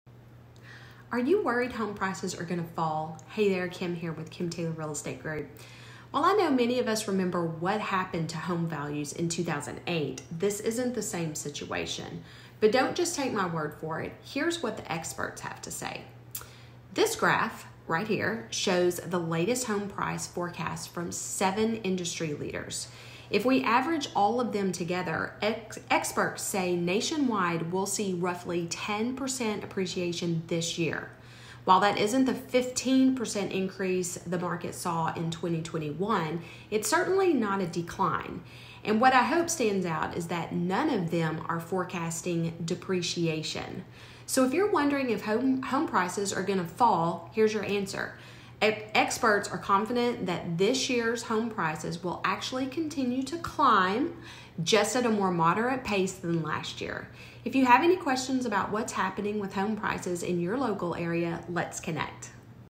August 18, 2022
Are home prices going to decline???  Take a listen!